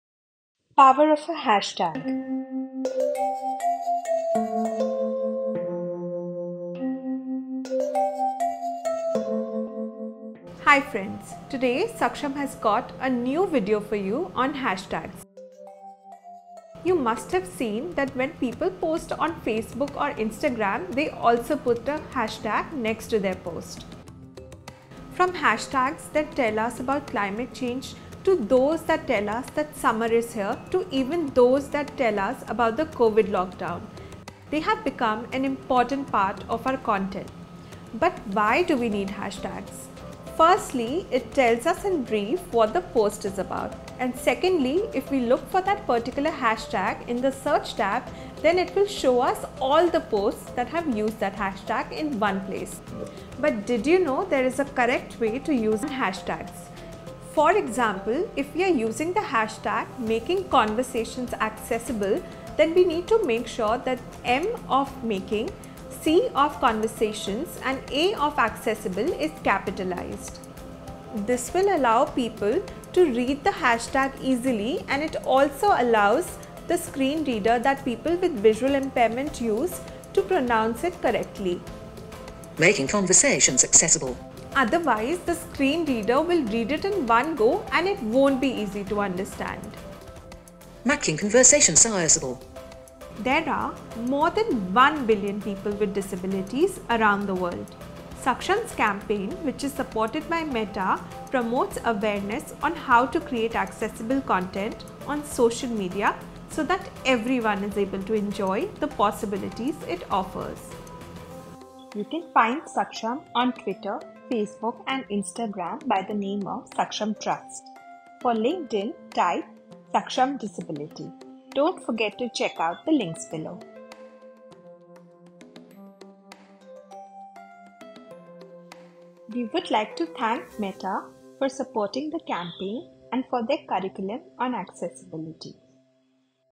5. Power of Hashtags 'How to Use CamelCase' - An initiative by Saksham supported by META
📢 Did you know that utilizing CamelCase in hashtags can improve accessibility for individuals with visual impairments?

By capitalizing the initial letter of each word, like BestFriendForever rather than bestfriendforever, hashtags become more readable and are pronounced more accurately by screen readers.

Today, let's delve deeper into this subject with our campaign titled 'Leveraging Social Media AccessAbility For All'.

 MORE ABOUT SAKSHAM!